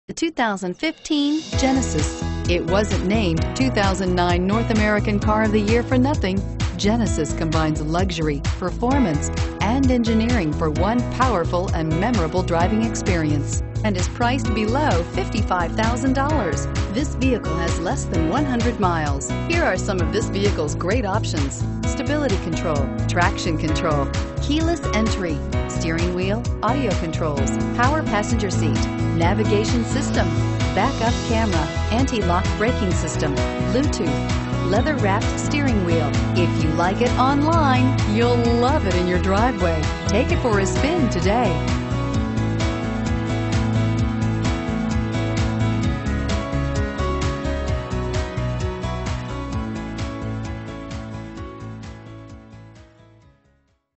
2015 Hyundai Genesis Irving TX HFU042803
2015 Hyundai Genesis 3.8L

Freeman Auto
1840 E Airport Fwy

Pick up this  2015 Hyundai Genesis, available today at Freeman Auto. Featuring Automatic transmission, it has 13 miles. This could be the one you've been searching for.